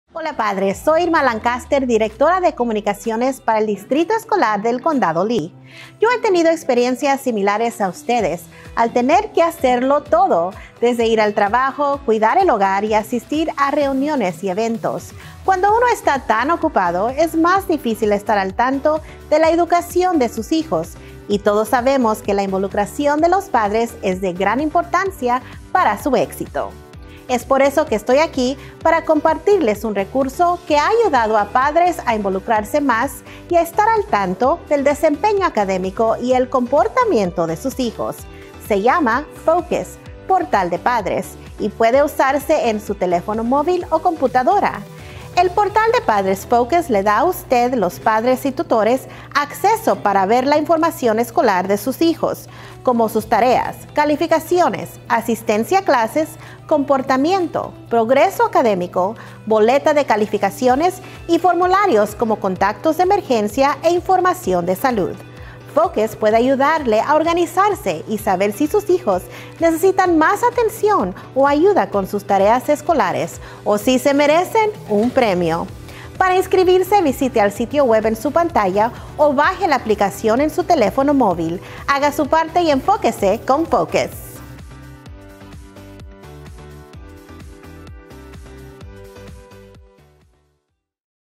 FOCUS Parent Portal - Spanish
Esta en busca de una manera más fácil de estar al tanto de la educación de sus hijos? El Distrito Escolar del Condado de Lee invita a padres y tutores a que se inscriban para el Portal de Padres FOCUS, que le ayuda a revisar las calificaciones, asistencia a clases, y mucho más.

Para más información o para crear una cuenta del Portal de Padres FOCUS, visite